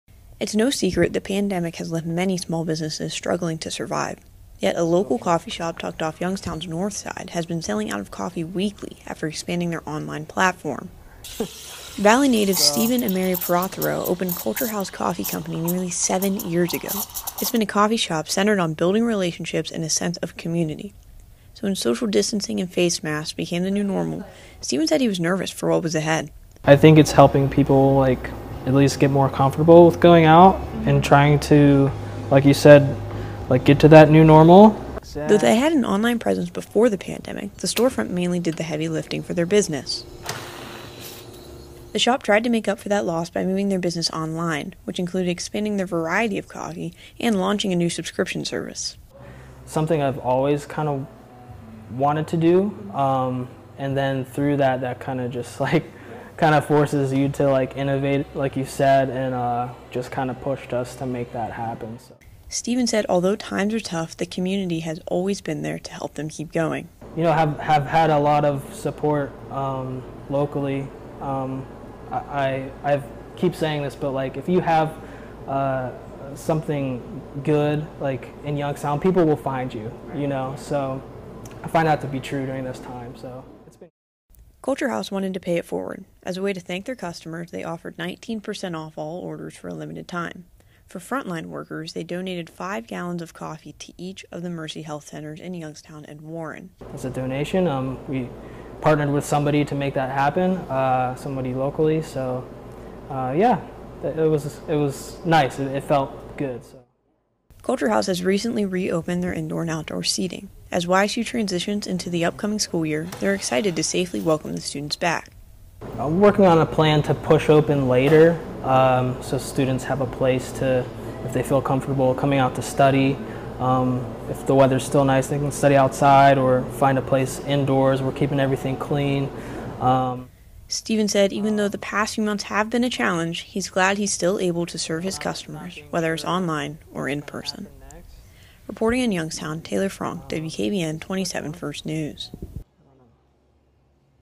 Local coffee shop leans on community support throughout pandemic
It’s no secret the pandemic has left many small businesses struggling to survive. Yet, a local coffee shop off Youngstown’s Northside has been selling out of coffee weekly after expanding their online platform.